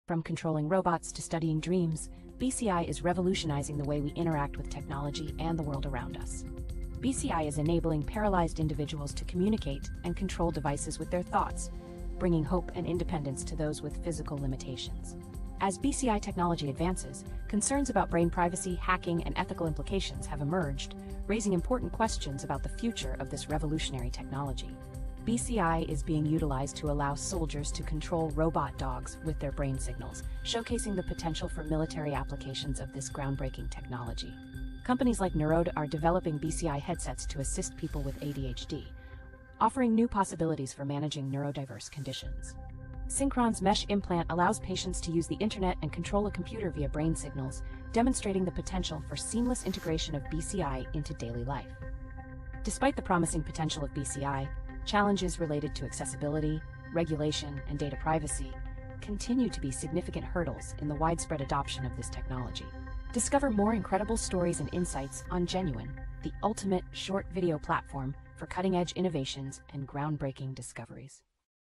Elon Musk’s Neuralink left behind in Brain chips: the Sydney researchers miles ahead in innovation
Brain chips: the Sydney researchers ‘miles ahead’ of Elon Musk’s Neuralink

Brain-computer interface (BCI) technology is no longer just a concept in science fiction movies like Ready Player One and The Matrix. On Earth, BCI is being used to help paralyzed individuals communicate, study dreams, and control robots. Elon Musk's company Neuralink has implanted a computer chip in a human, enabling them to control a computer mouse with their thoughts. However, concerns about brain privacy, hacking, and ethical implications have been raised. Other BCI advancements include a project at the University of Technology Sydney allowing soldiers to control a robot dog with their brain signals, and a headset by Sydney company Neurode to help people with ADHD. Synchron, a company in Australia and New York, has developed a mesh implant allowing patients to use the internet and control a computer via brain signals. Despite the promising potential of BCI, concerns about accessibility, regulation, and data privacy persist.